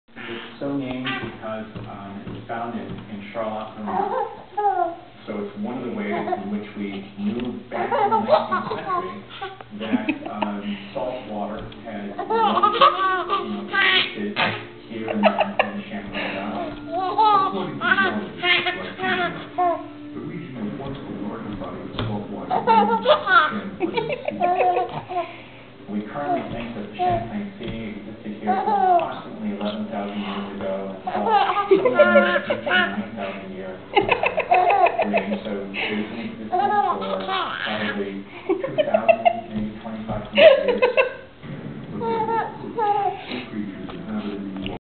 Tug of war
Hayley playing tug of war with mom and a rag. Just gotta love her laugh!